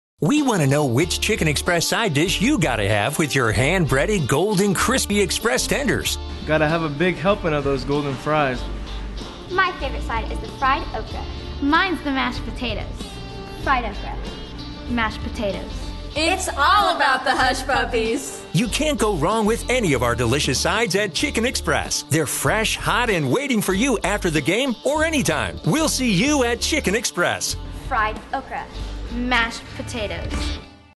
Chicken Express and the Smithson Valley Rangers
What side dish do Smithson Valley Rangers Fans gotta have with their Chicken Express Tender combos?
Executive Producer - Lori & Kenny Brown
Writer Director - Bo Wich